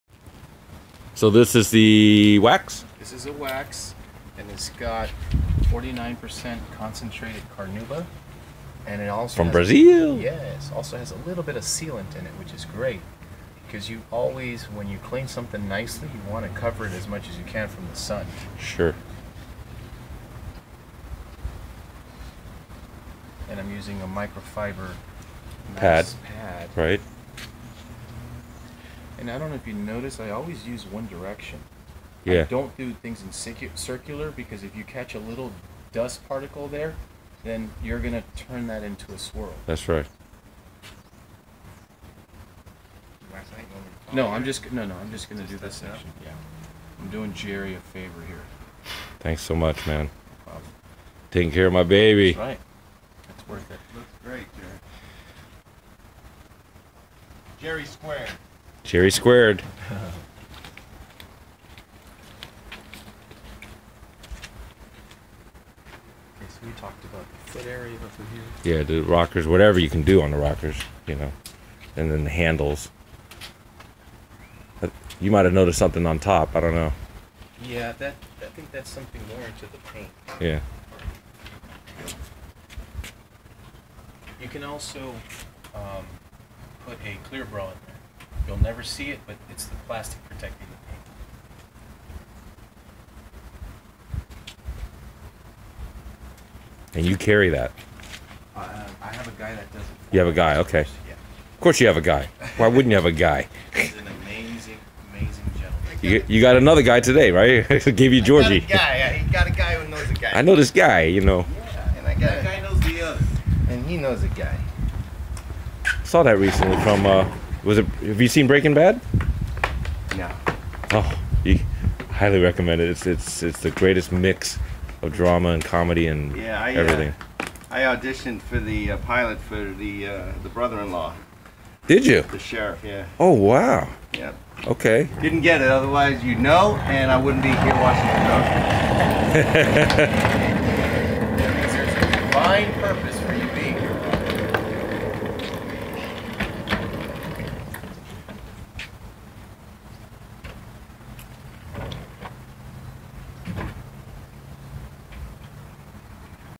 MVP Auto Detail touch up wax 2008 Acura TL-S
MVP Auto removing light scratches with Carnauba wax on 2008 Acura TL-S